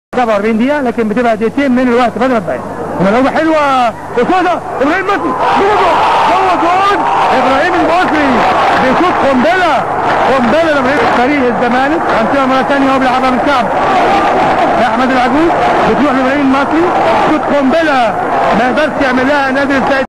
هدف ابراهيم المصري في مرمي الزمالك - الدور الثاني لموسم 1997/1996
هدف ابراهيم المصري في مرمي الزمالك - الدور الثاني لموسم 1997/1996  علي ستاد بورسعيد و قد اقيمت تلك المباراة في شهر يوليو عام 1997 و انتهت بالتعادل بهدف واحد لكل فريق